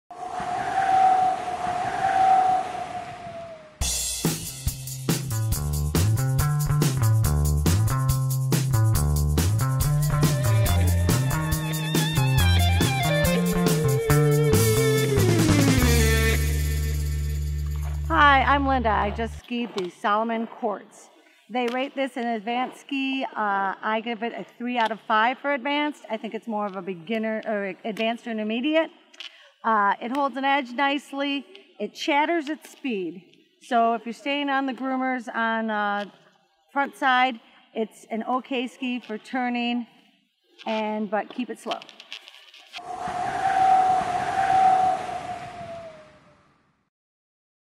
Linda's Review-Salomon Quartz Skis 2015-Skis.com
The new Salomon Quartz is a perfect ski for the advanced to expert skier that is looking for a ski that will make smooth and lively turns on the hard pack. Built with Salomon's Light Density Wood Core with a single Ti Layer the Quartz give you a stable ski that will not be overly heavy, and still have a very quick and lively feel. The new All Terrain Rocker 2.0 has a little more Rocker in the tip to engage your turns quicker, and dampen out vibrations. Slight tail rocker has been added to release you out of your turns effortlessly. RKS (Rocker Stabilizer) makes the Quartz ski smoother without any extra tip wobble by adding a 3D layer of composite fiber in the contact zones, and improves your control. Salomon also has the X-Chassis that has extra reinforcement to give you more torsional stiffness for better edge hold in ice or firmer slopes. If you are looking for a ski that will crush it down the groomers with a light and lively feel that will be stable no matter how fast you ski, the Salomon Quartz will not disappoint you.